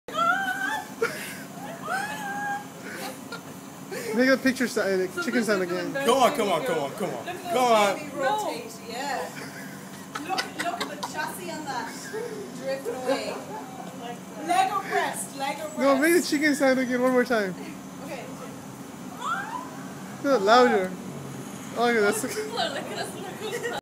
RCSI Rotisserie Chicken Dance
A rare art form that is almost extinct but skillfully performed here by our very own RCSI Chicks :p